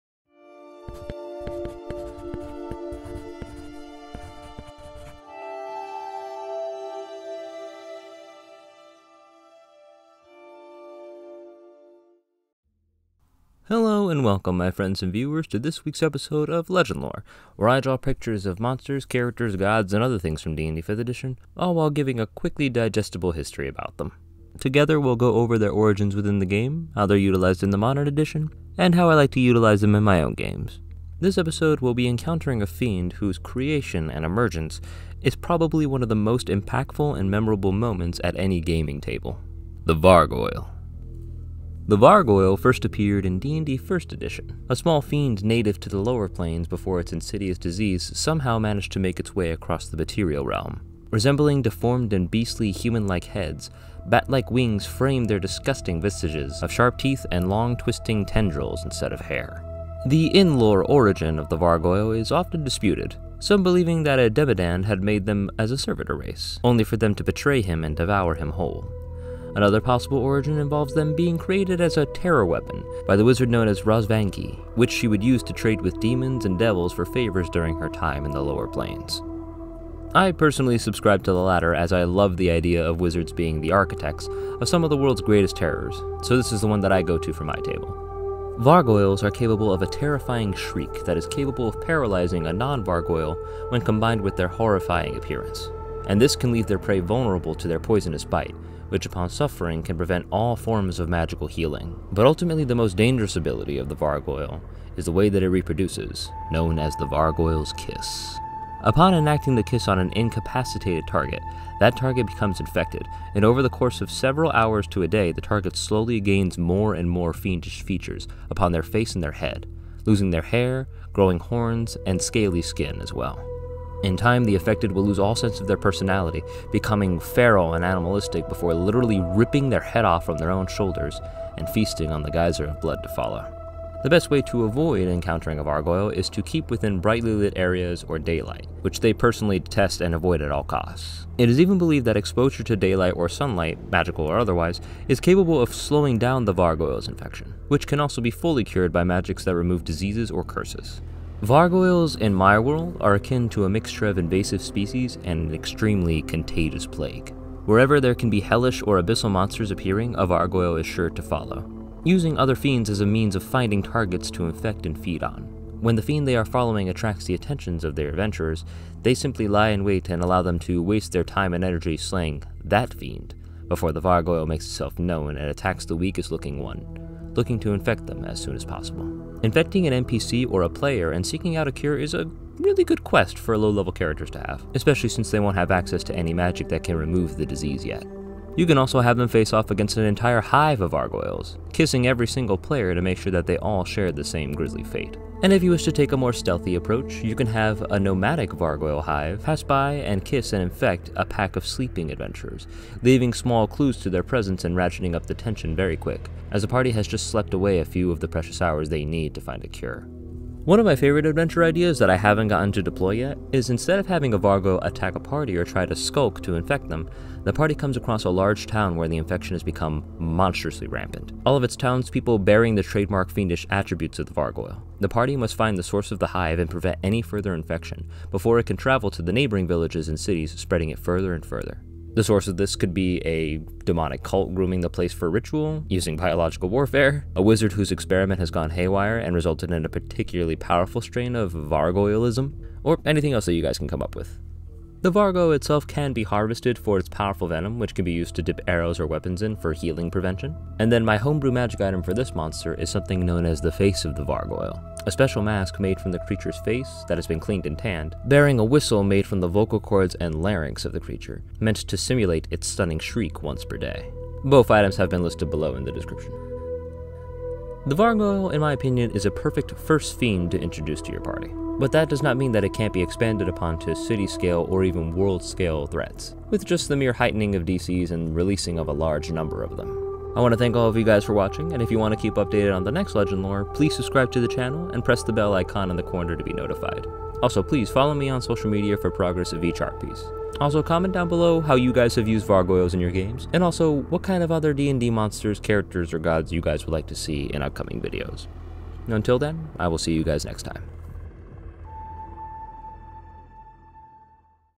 D&D Legendlore: The Vargouille | D&D 5th Edition Creature Breakdown
Hello and welcome friends to Legendlore, where I draw pictures of gods, monsters, and characters from D&D while giving a small but dense history about them! This episode, we will be encountering a fiend whose emergence is probably one of the most impactful and memorable moments for any gaming table: the Vargouille.